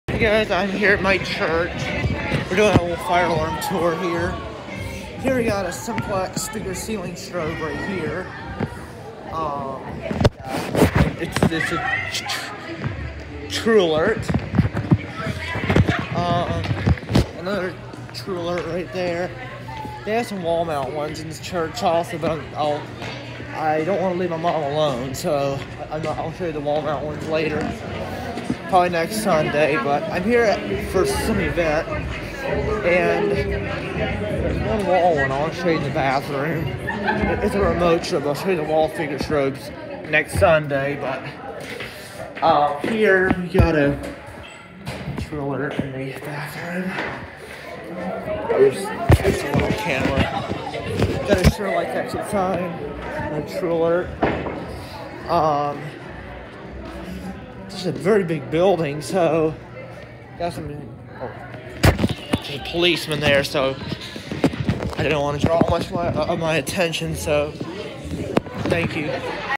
Fire alarm tour at my church
Crossroads fellowship church Raleigh NC
Pull stations simplex dual action
Devices simplex ceiling truealert speaker strobes
Simplex true alert remote wall strobes
Simplex true alert wall speaker strobes